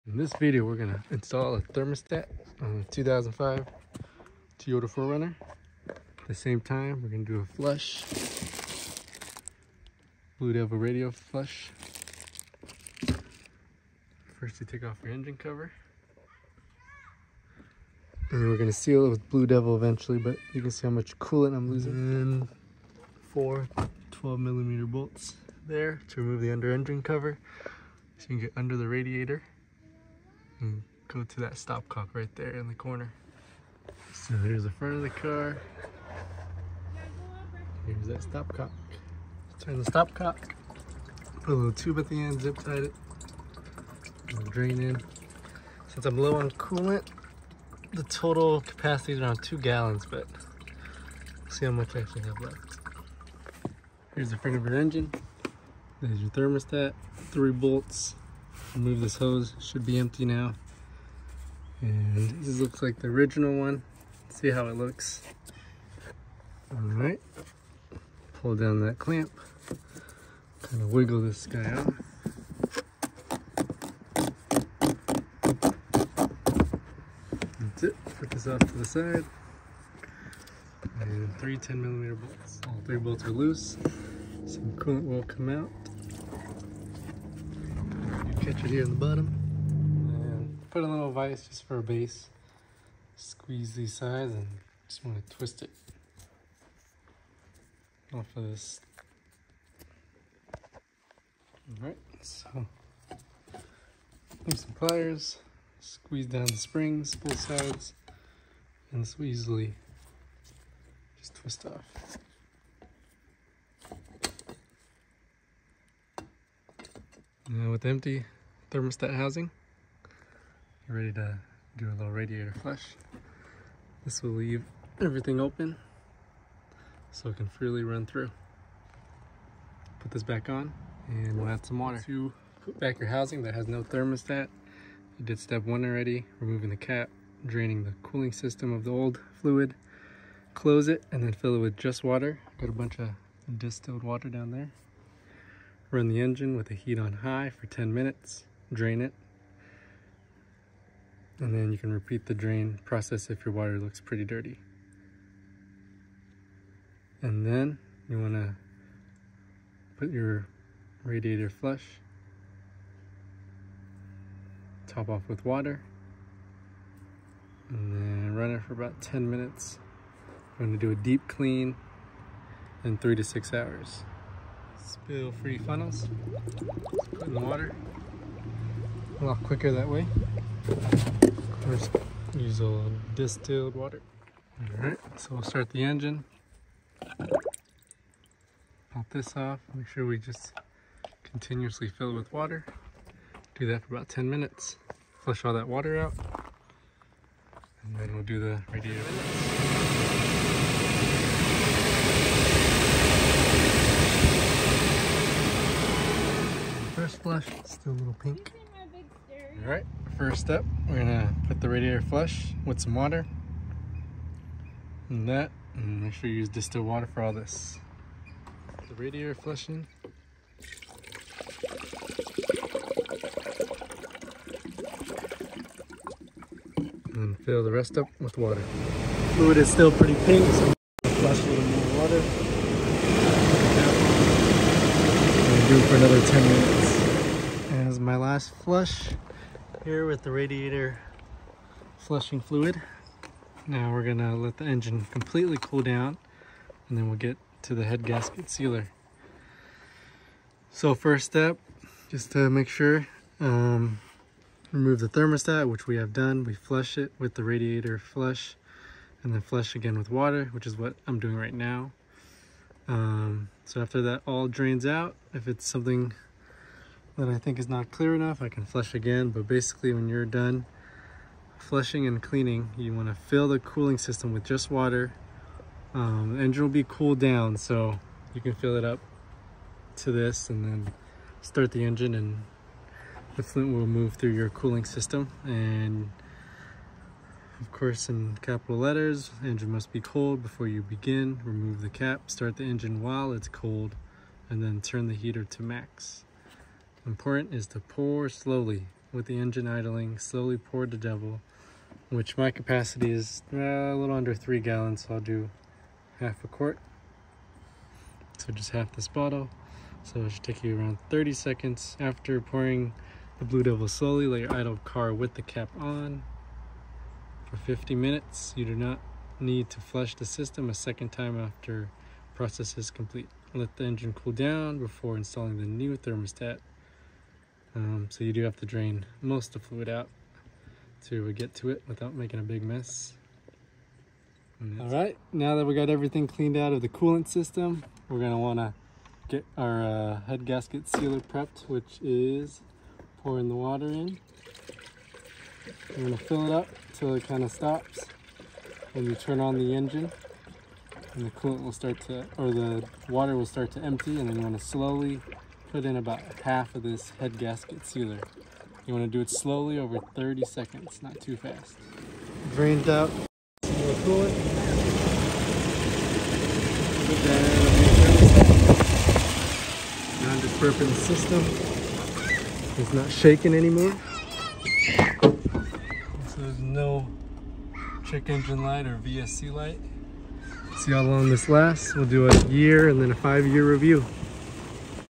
How to use BLUE DEVIL Head Gasket Sealer - Doesn’t clog your system !!
Part 3 - Show you how to easily seal your leaking head gasket and cracked heads. You will need to remove your thermostat for this procedure but worth saving thousands of dollars.

Amazon items I used in this video -